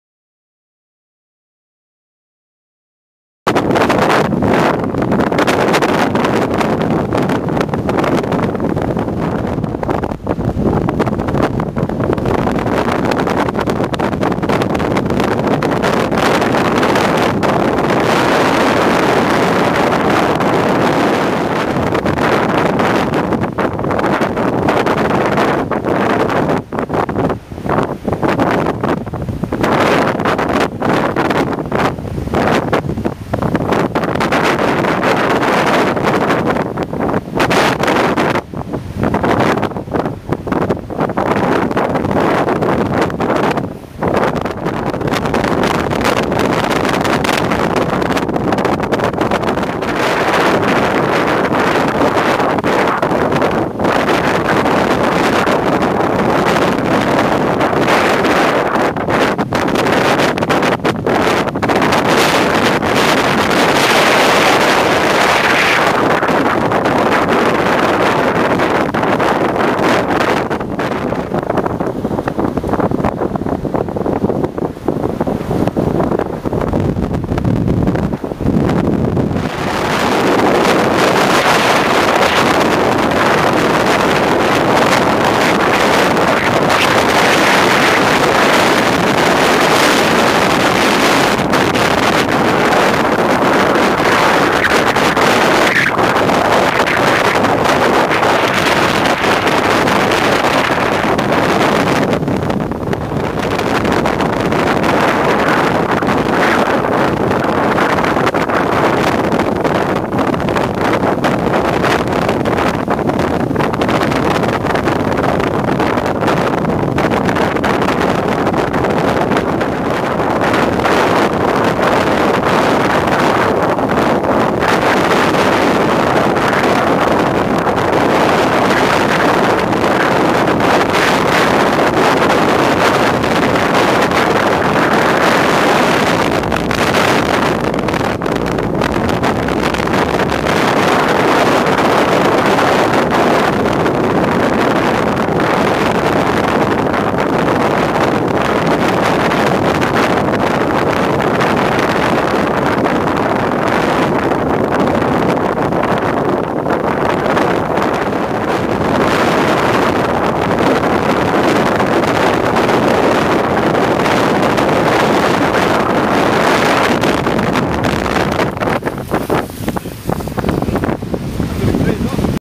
GIORNATA DI BORA A SOTTOMARINA 20/06/09 (parte 2)
Ecco la seconda uscita nel pomeriggio, con ancora 30 nodi con rafiche di 35 nodi alcune volte anche 37 !!!